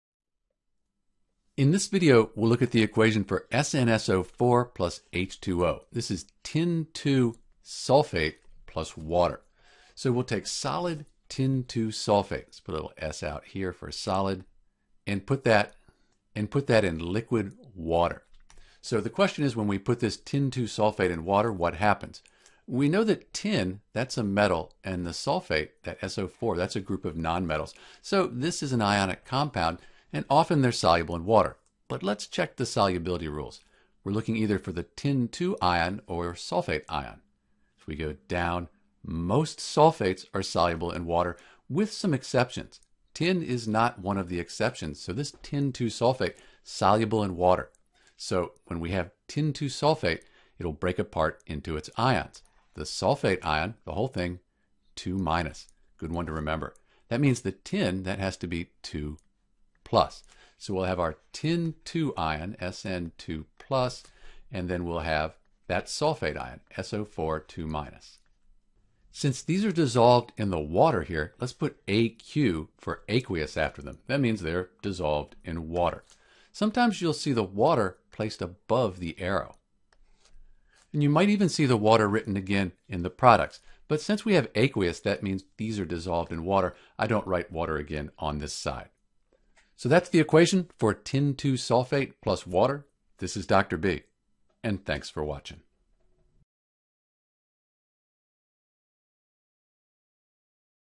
Equation for SnSO4 + H2O
In this video we will describe the equation SnSO4 + H2O and write what happens when SnSO4 is dissolved in water.

When SnSO4 is dissolved in H2O (water) it will dissociate (dissolve) into Sn 2+ and SO4 2- ions. To show that they are dissolved in water we can write (aq) after each. The (aq) shows that they are aqueous – dissolved in water.

The equation for SnSO4 ( Tin (II) sulfate and H2O sometimes isn’t considered a chemical reaction since it is easy to change the Sn 2+ and SO4 2- back to SnSO4 (just let the H2O evaporate). At the same time, the SnSO4 is a very different substance than Sn 2+ and SO4 2-.

Knowing the solubility rules can help you determine whether SnSO4 will dissolve in water and dissociate into ions